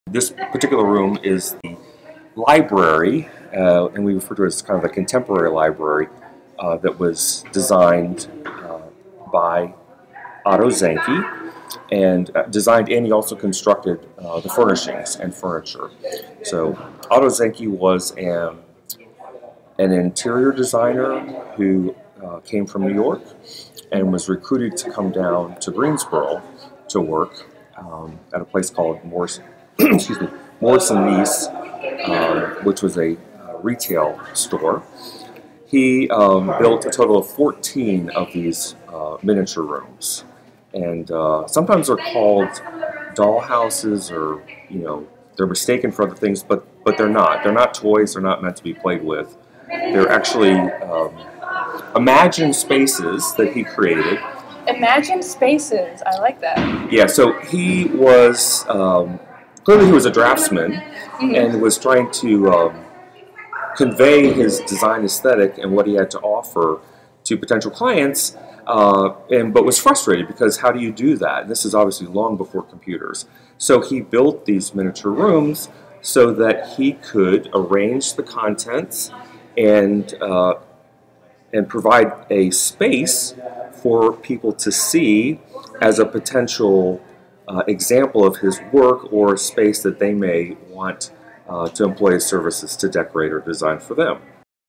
Zenke Miniature Rooms: Modern Library
Jon Zachman, Curator of Collections at the Greensboro History Museum, shows us one of Otto Zenke's miniature rooms.

During his lifetime, Otto Zenke designed and built fourteen miniature rooms, several of which were exhibited in New York and other American cities. Following Zenke’s death in 1984, the twelve surviving miniature rooms were donated to the museum by his niece Julie Bedell. Three are currently on exhibit: the Bedroom, Modern Dining Room, and Modern Library.

Inspired by Mrs. James Ward Thorne’s miniature rooms, Zenke created elegant designs that reflect his love of late 18th-century English interiors. Rather than using the standard scale of one inch equals one foot, Zenke preferred the unusual one and one-half inches to a foot.

Born in Brooklyn, New York in 1904, Otto Zenke studied interior design and started his career at B. Altman & Co. in New York before moving to Greensboro, North Carolina, where he worked as chief decorator at Morrison-Neese Furniture Company during the late 1930s and 1940s. Zenke opened his own design studio and showroom in 1950 called Otto Zenke, Inc. He designed and decorated primarily residential properties for another three decades and eventually opened offices in Palm Beach, Florida and London.